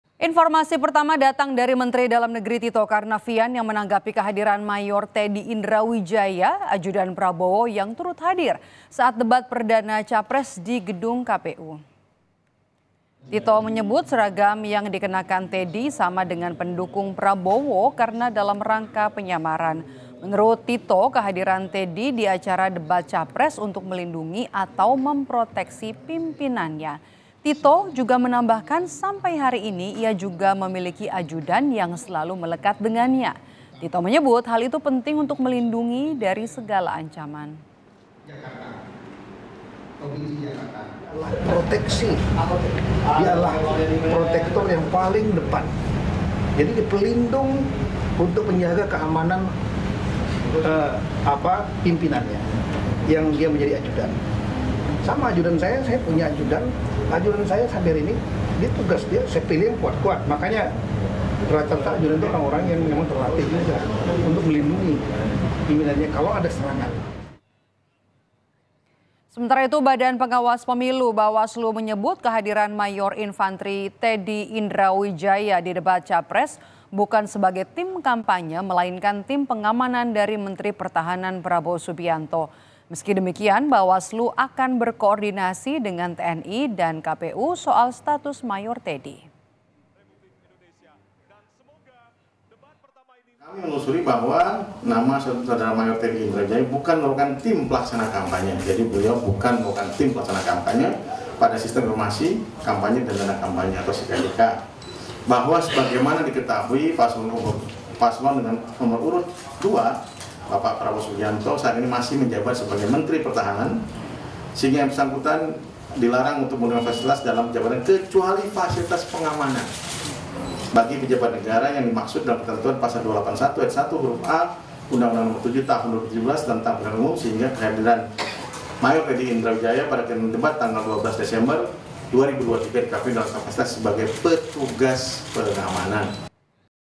Ajudan Prabowo Mayor Teddy Hadir di Debat Capres, Mendagri: Di Kampanye untuk Melindungi
KOMPAS.TV - Ajudan Prabowo Mayor Teddy Hadir di Debat Capres, Mendagri: Di Kampanye untuk Melindungi

Kompas TV
Independen | Terpercaya